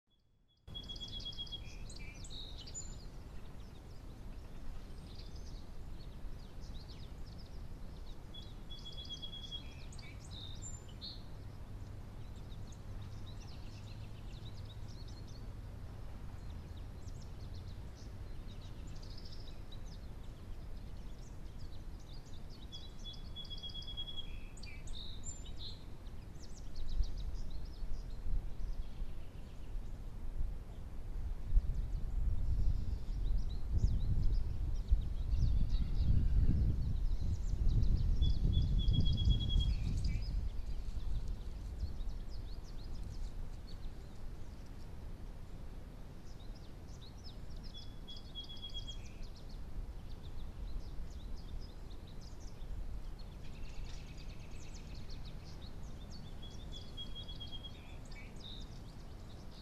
Groundhog
Looks healthy and pretty alert after hybernation